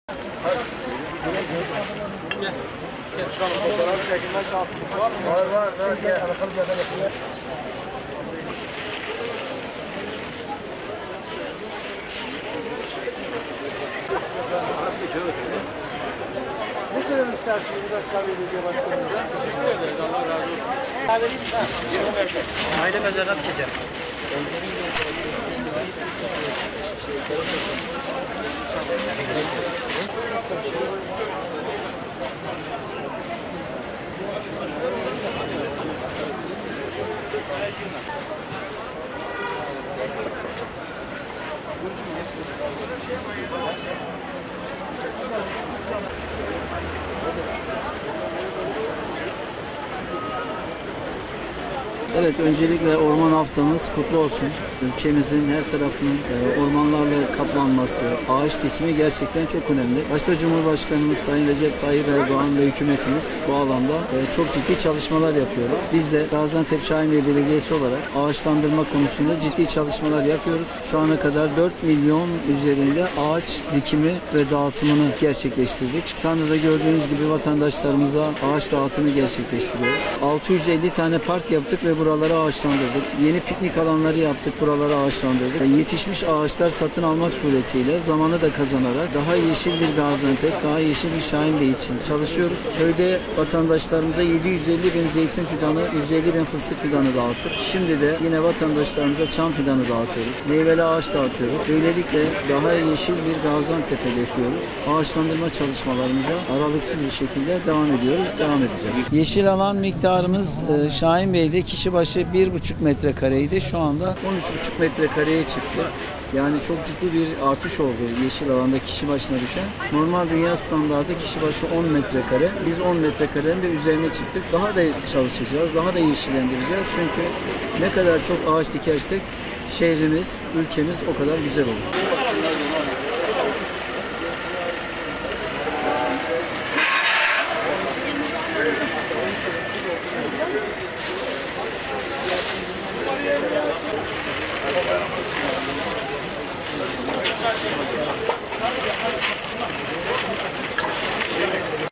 Şahinbey’de 150 bin çam fidanı dağıtıldı
Gaziantep'in Şahinbey ilçesinde Orman Haftası nedeniyle 150 bin adet çam fidanı dağıtımı gerçekleştirdi.
Her yıl geleneksel olarak fidan dağıtımı gerçekleştiren Şahinbey Belediyesi bu geleneğini bu yılda Orman Haftası’nda sürdürmeye devam etti. Kent’in en yoğun olduğu yerlerden biri olan Balıklı Parkı’nda vatandaşlara 150 bin adet çam fidanı dağıtımına başlayan Şahinbey Belediye Başkanı Mehmet Tahmazoğlu, daha yeşil bir Gaziantep ve Şahinbey için fidan dağıttıklarını ifade etti.